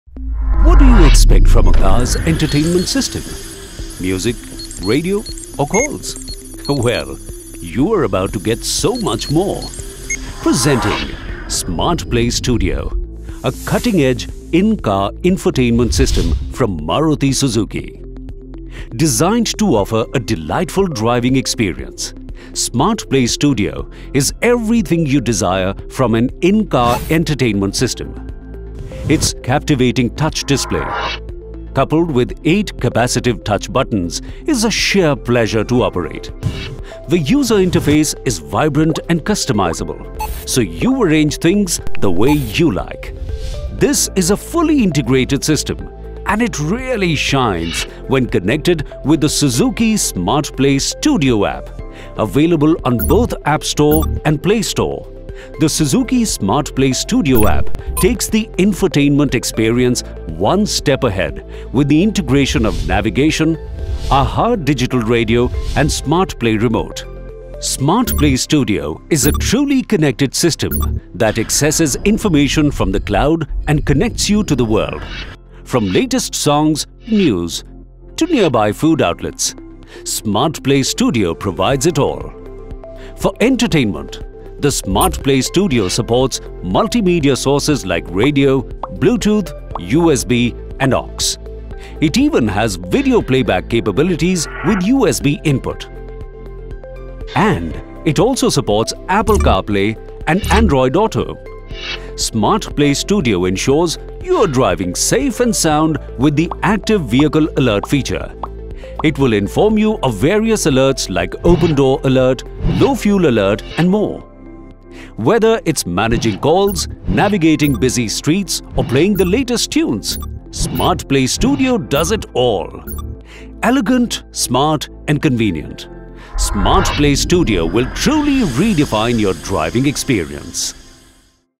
MarutiSuzuki New Smartplay Infotainment System - Smartplay Studio
Maruti Suzuki Smartplay Studio, a better, safer and smarter approach to in-car entertainment. This new-age, connected infotainment system connects your vehicle to the Cloud and provides a host of features. It not only enhances your driving experience with the latest music,  news update, weather report and restaurant recommendations but also informs you about your car’s status via vehicle alerts. Loaded with top-of-the-line features, Smartplay Studio is here to redefine what an in-car entertainment system can do.